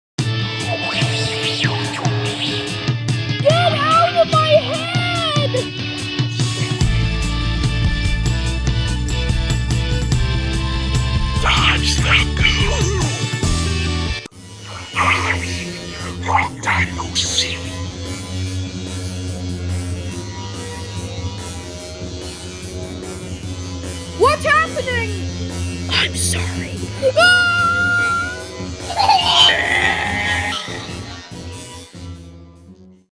pooky and chooky show season 2 tesser trailer👌
this is a tesserfor pooky and chooky show season 2 truth of space this show happens 2 year after the first session it is pretty cool Rudolph and joeys are in high school and maybe some surprises in the show after the events of the fist season the town will be a complete mess all buildings are a complete disaster it is a wreck but then soon after they get sucked in to space the learn the truth that's all you need to know about the pilot lets goo this is only the trailer. we want 2 hit 100 subs lets go!